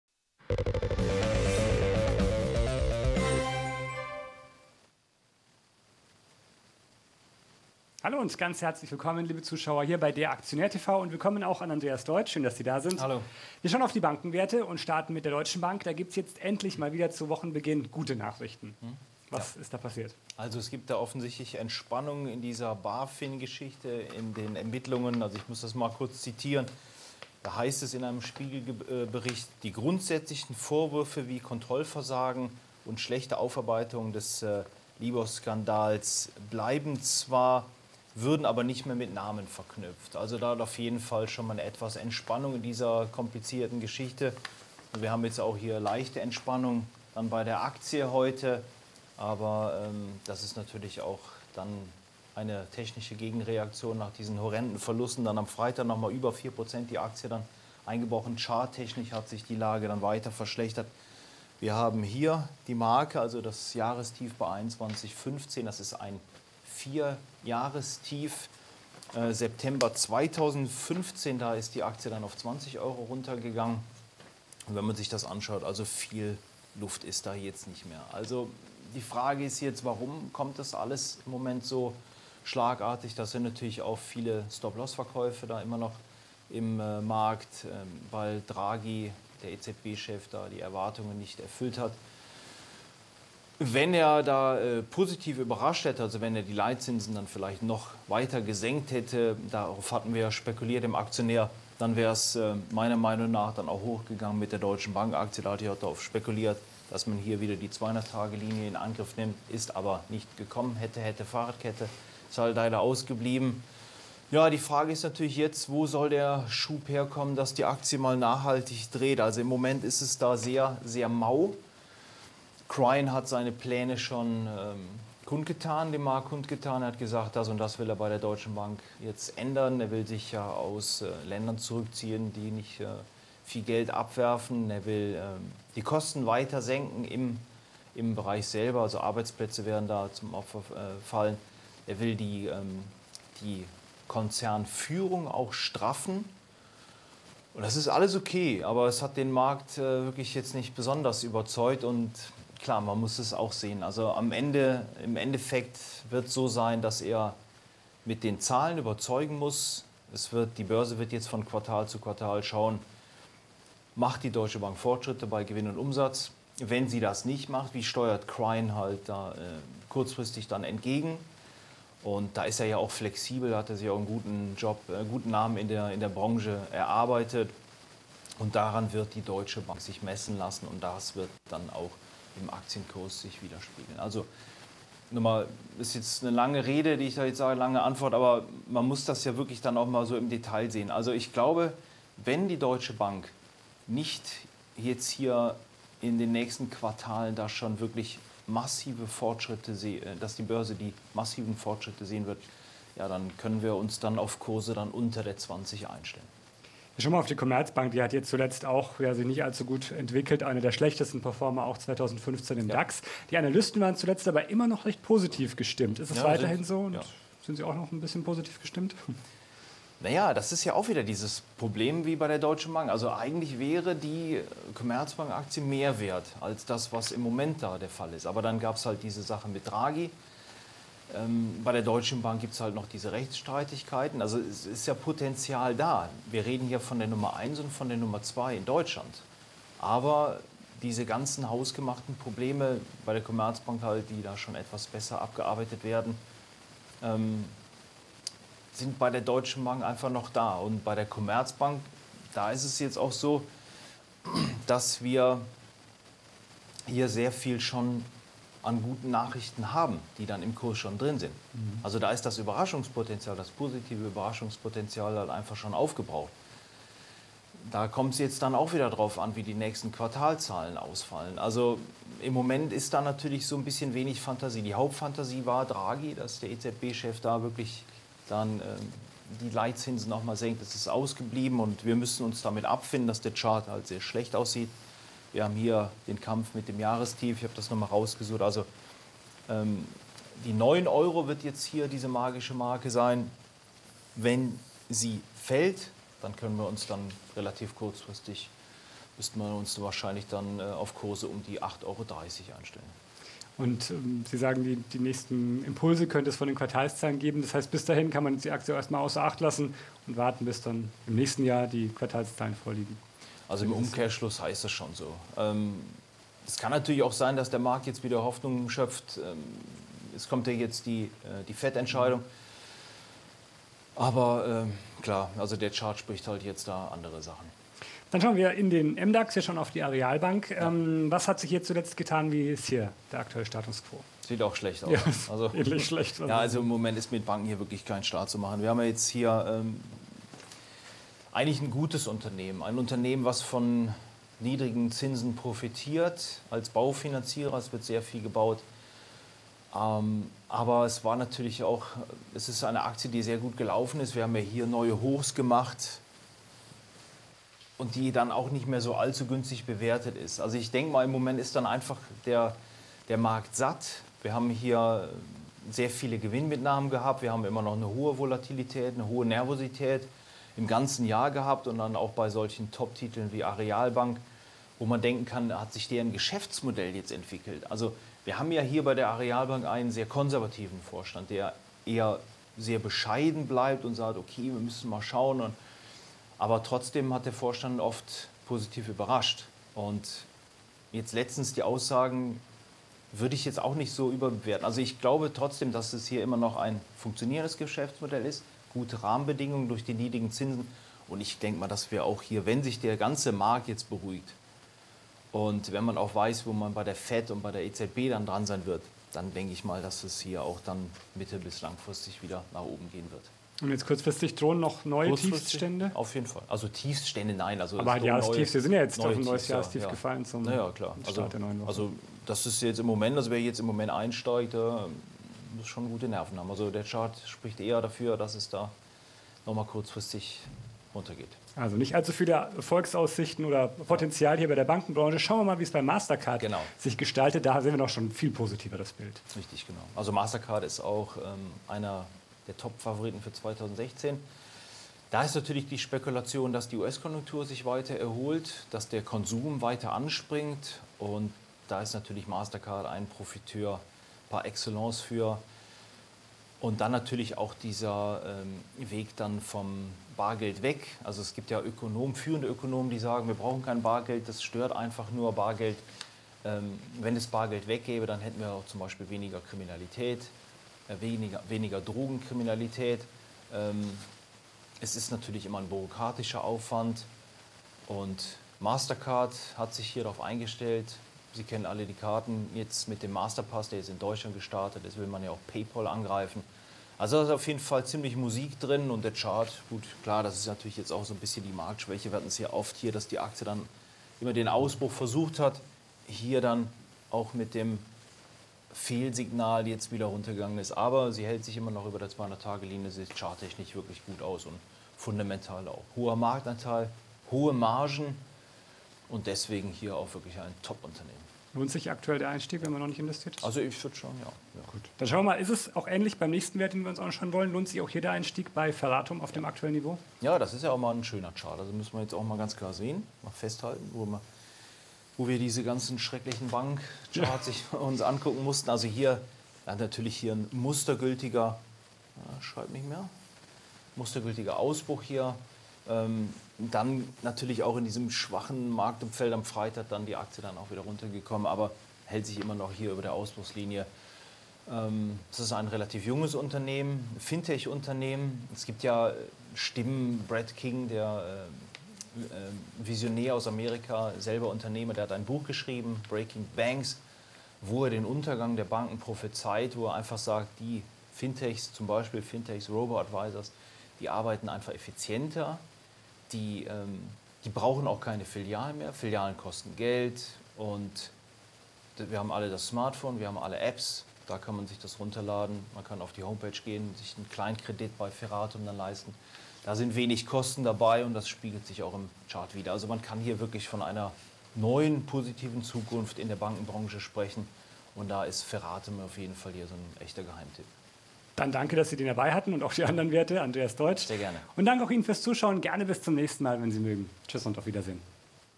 Finanztitel: Das sind die zwei Aktien des Schreckens
Andreas Deutsch vom Anlegermagazin DER AKTIONÄR blickt auf die Aktien von Deutsche Bank, Commerzbank, Aareal Bank, Mastercard und Ferratum.